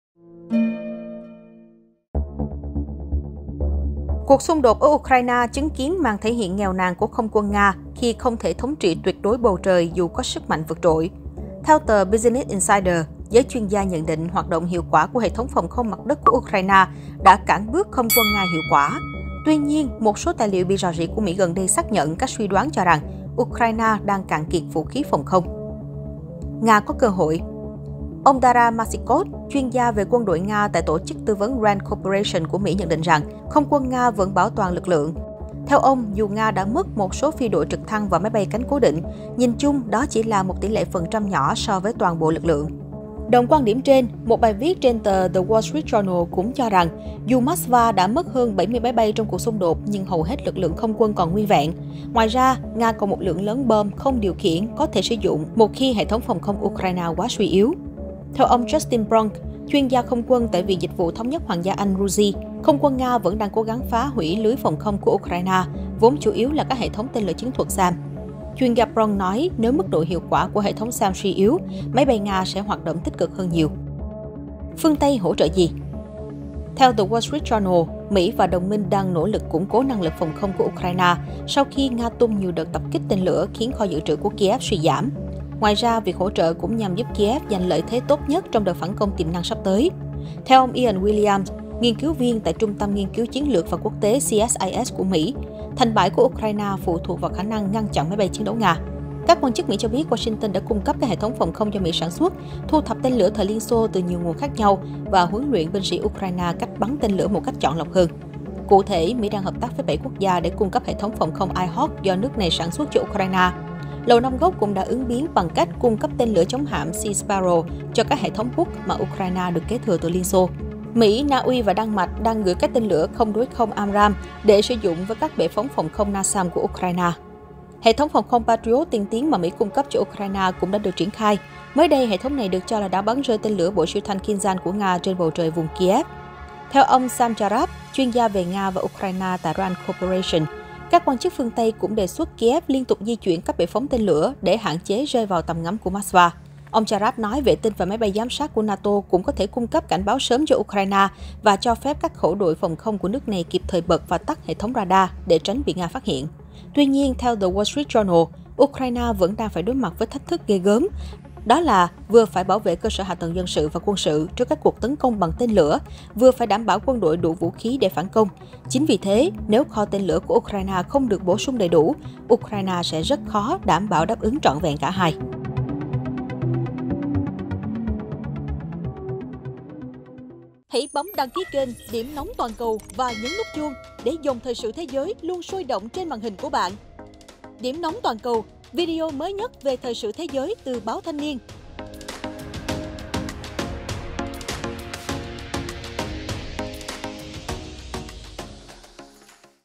Ukraine lộ điểm yếu phòng không, phương Tây gấp rút giúp khắc phục?
Trong khi hệ thống phòng không Ukraine lộ điểm yếu, phương Tây đã tăng viện trợ để ngăn đồng minh thất thế trước lực lượng không quân Nga, vốn gần như vẫn còn nguyên vẹn.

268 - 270 Nguyễn Đình Chiểu, Phường Võ Thị Sáu, quận 3, TP HCM